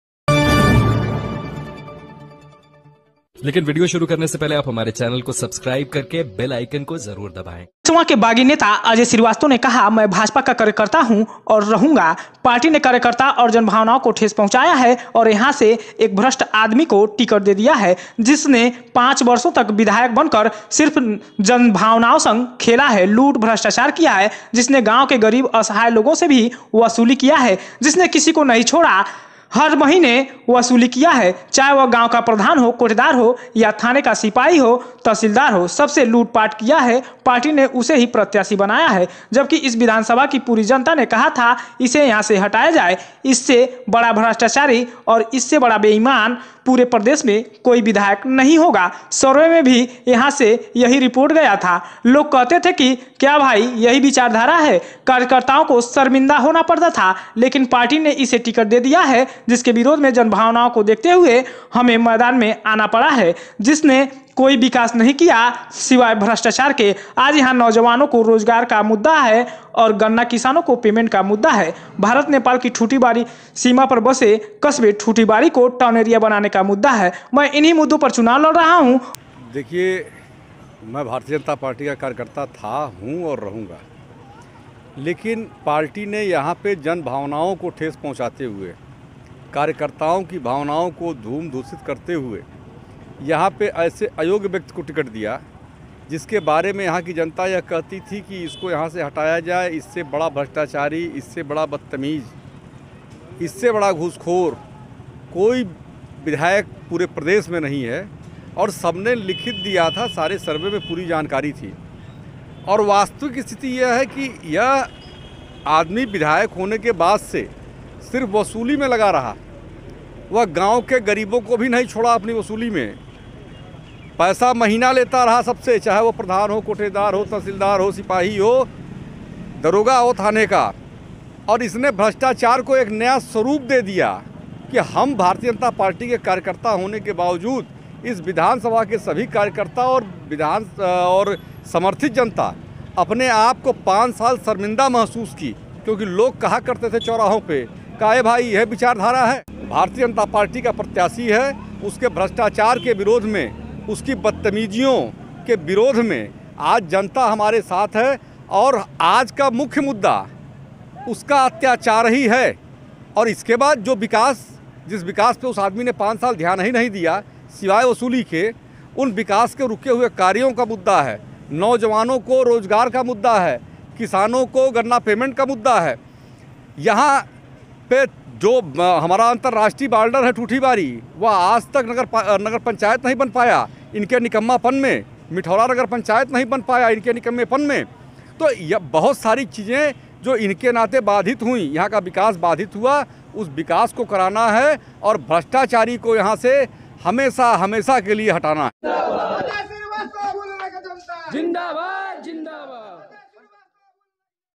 siswanभाजपा बागी नेता अजय श्रीवास्तव ने खोली विधायक की पोल कहा थाना,प्रधान,कोटेदार से करता है वसूली
@सैटेलाइट @ इंटरनेट इंडिया @SATELLITE NEWS @LIVE SATELLITE STUDIOS @satellitegrid.com @4tv News [ Satellite ] @satellitenewsservice